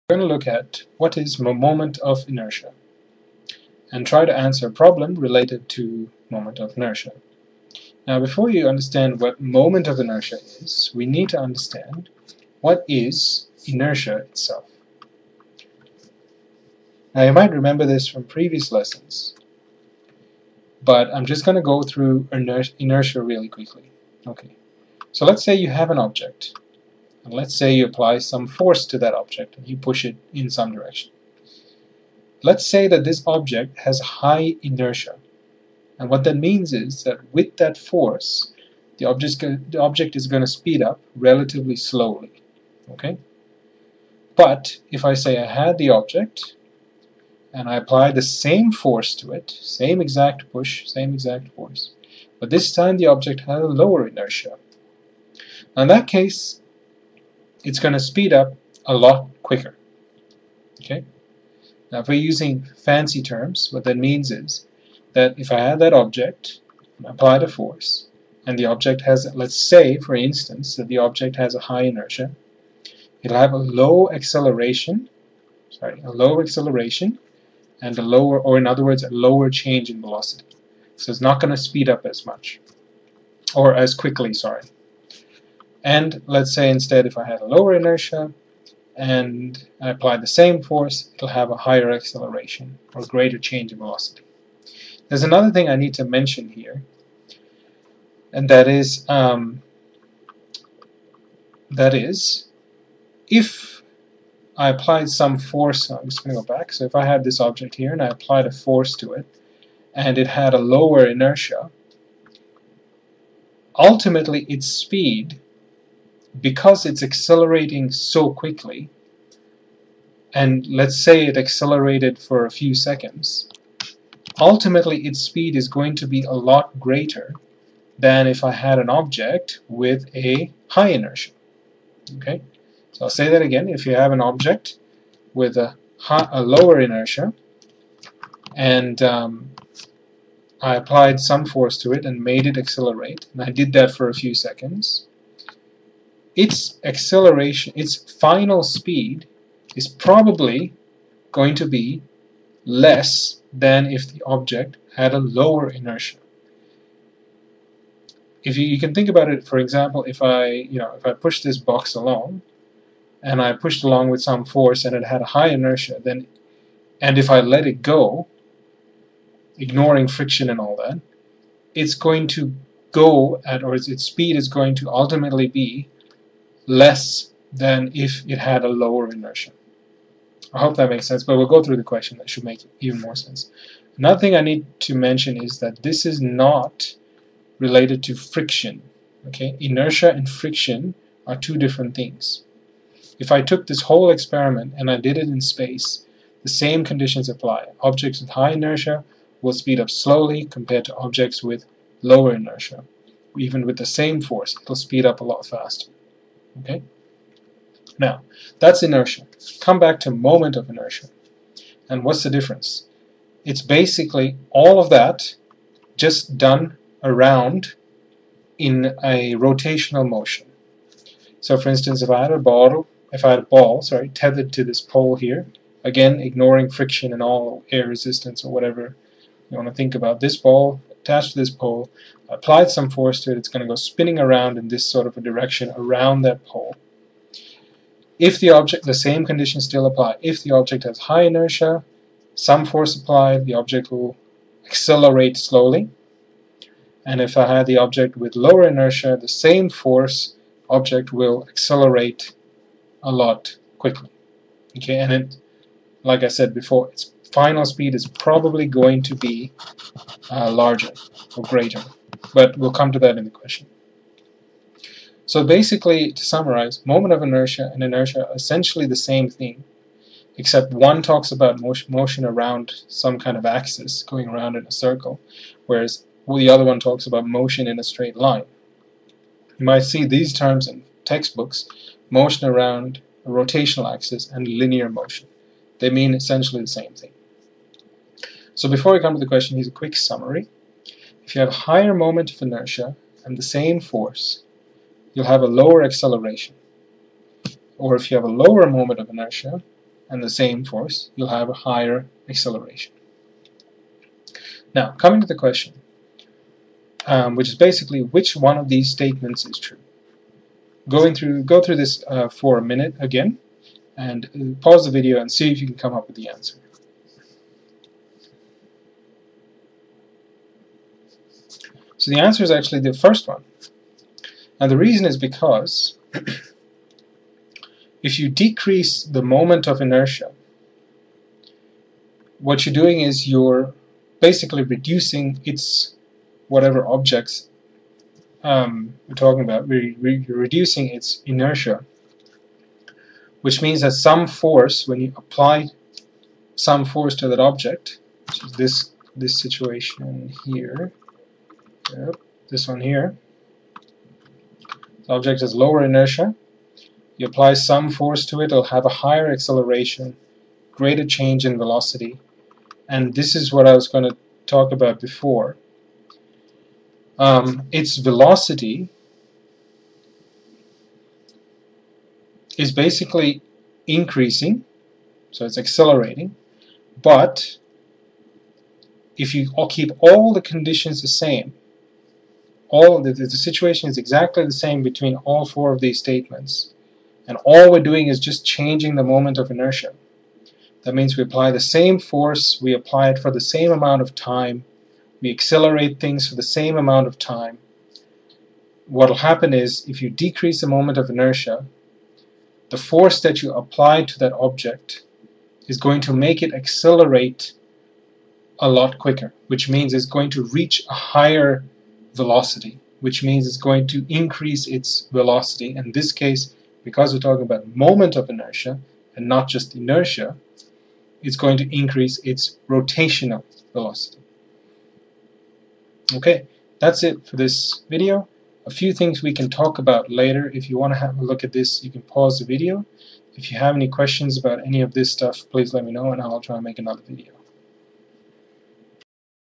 What is moment of inertia? [Basic concept only]
Video explaining the basic meaning of moment of inertia. Further videos on how to solve problems mathematically are to come. This just provides a qualitative explanation of MOI.

--- MEDIA SOURCES ---
Crate